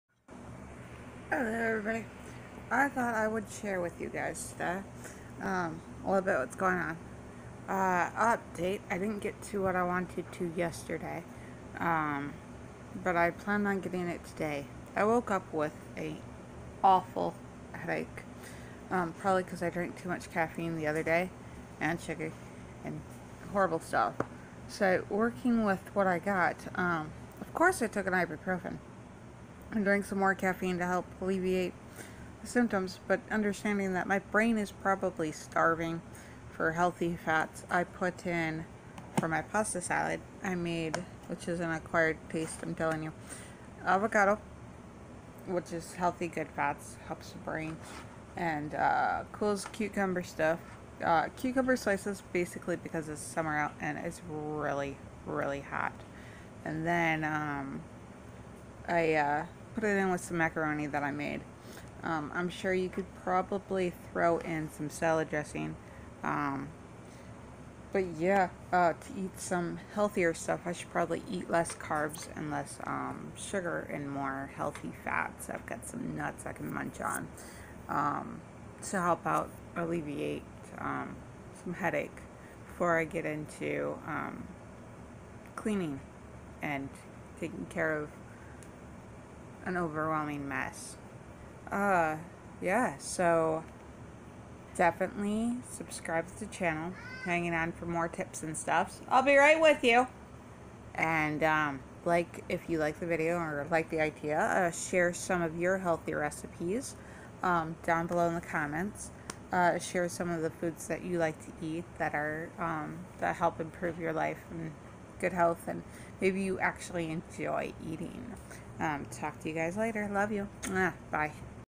tipa on brain healthy eating
sharing a healthier lunch idea about healthy fats and foods